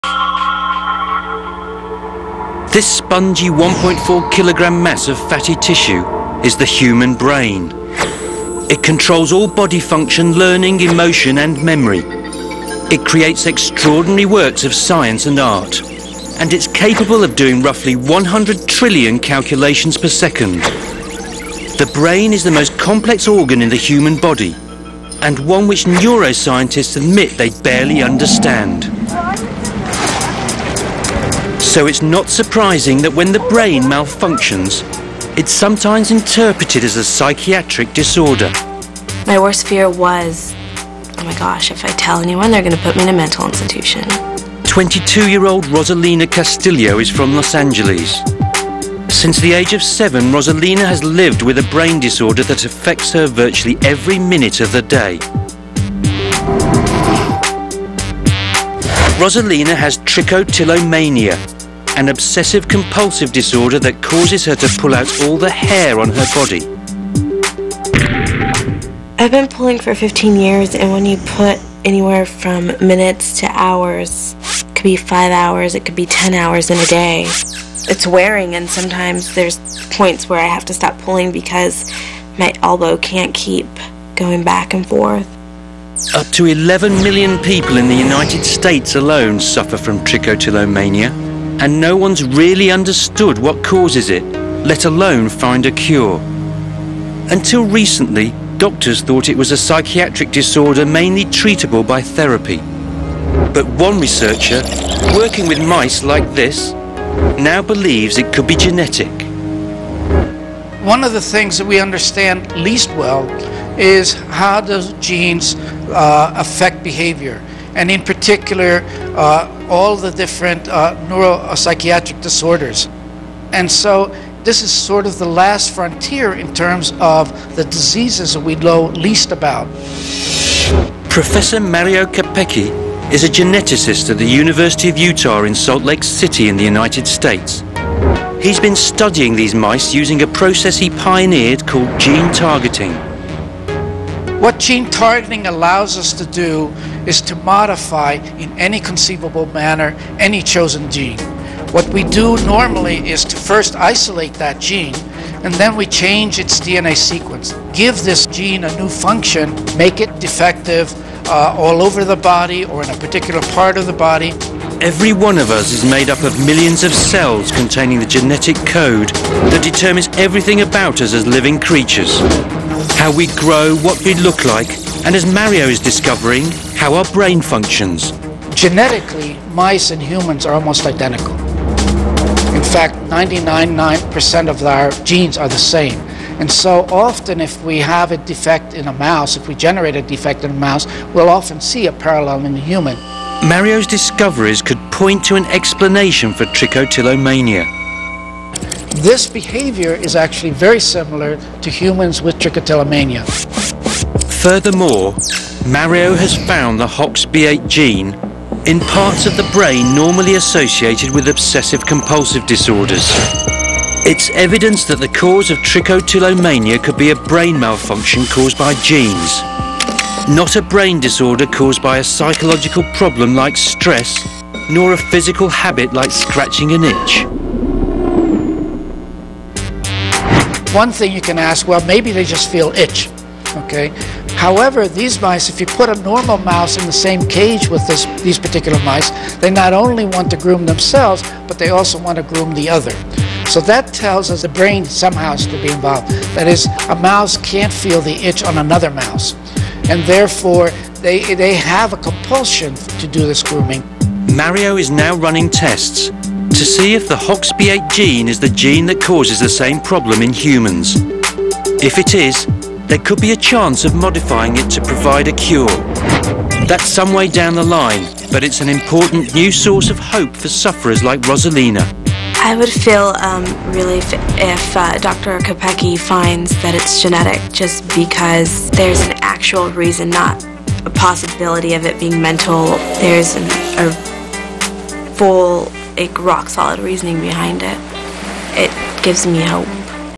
大腦奧秘--拔毛強迫症
先天基因異常與後天環境觸發